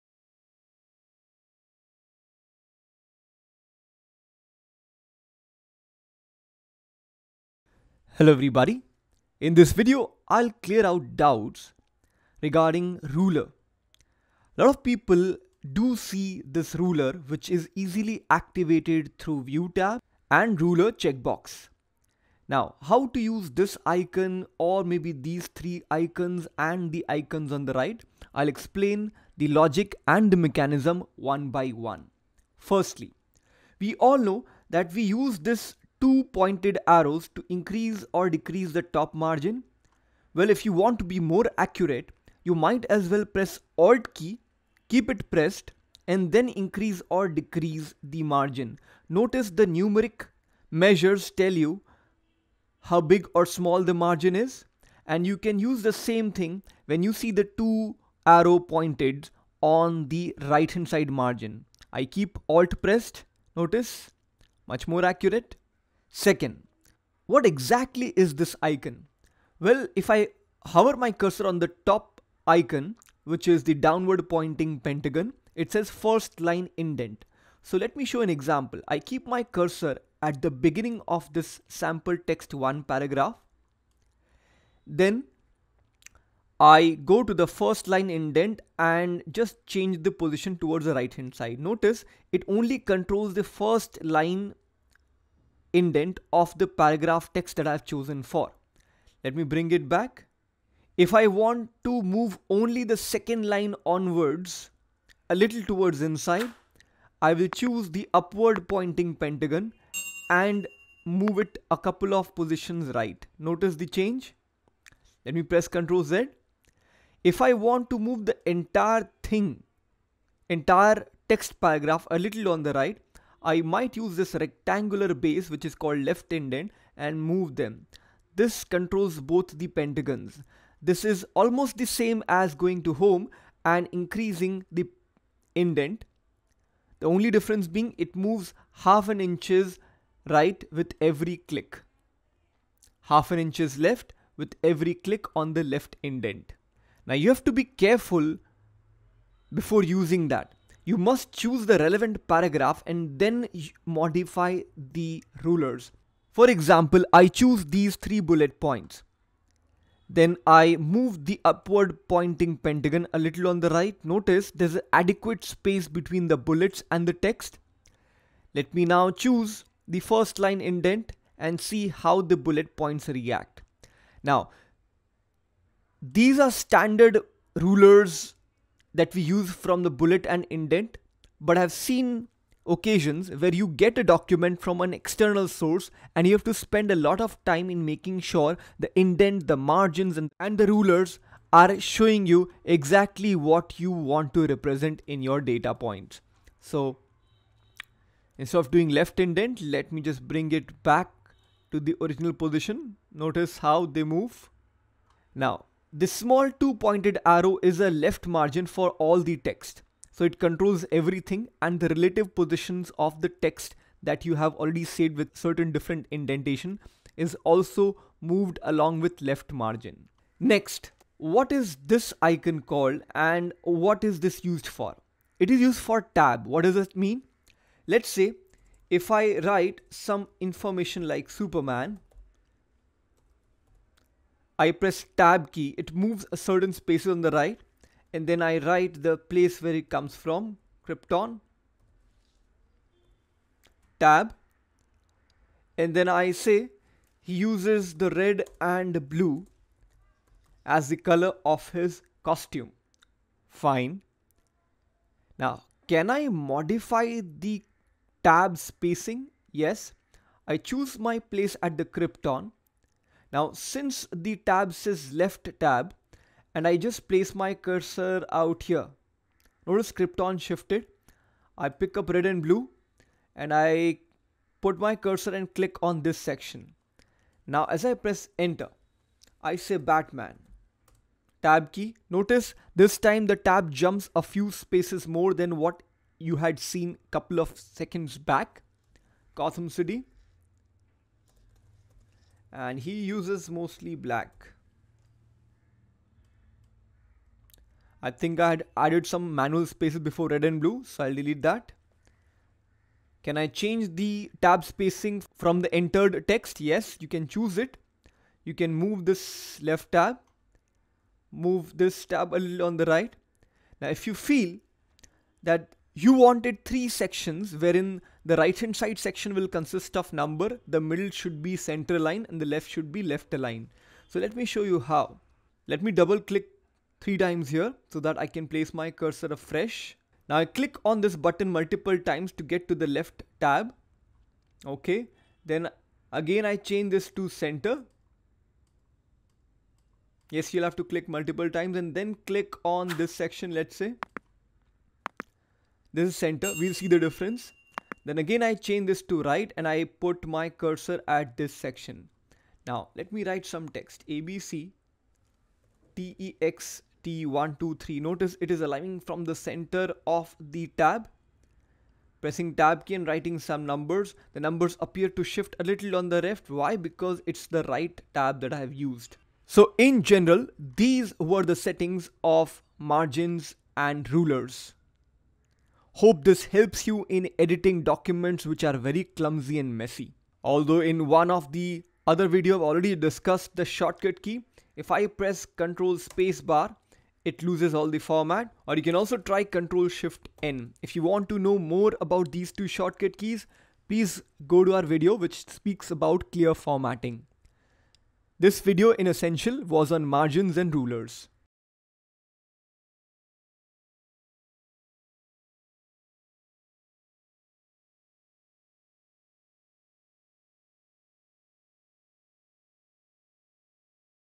Understanding Rulers and Margins in detail | MS Word Tutorial
Learn Understanding Rulers and Margins in detail in MS Word

We Covered the following topics:
00:00 Introduction to Rulers and Margins in MS Word
00:08 How to use Rulers in MS Word
00:23 How to use Margins in MS Word